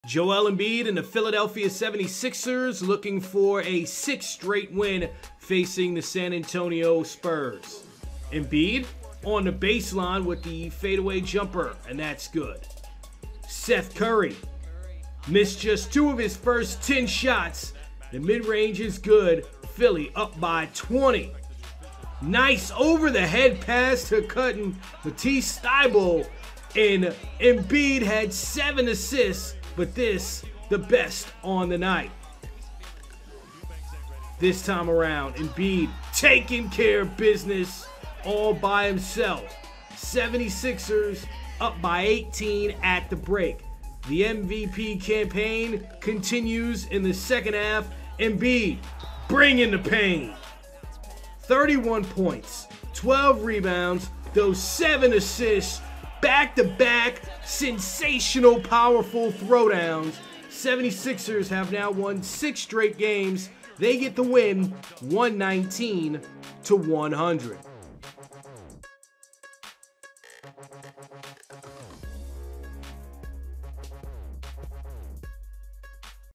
Game Recap: 76ers 119, Spurs 100
The 76ers defeated the Spurs, 119-100. Joel Embiid led the 76ers with 31 points, 12 rebounds and 7 assists, his 6th consecutive game with 30+ points. Tobias Harris added 23 points, 7 rebounds and 5 assists for the 76ers in the victory, while Dejounte Murray tallied 27 points, 5 rebounds and 9 assists for the Spurs in the losing effort. The 76ers improve to 22-16 on the season, while the Spurs fall to 15-23.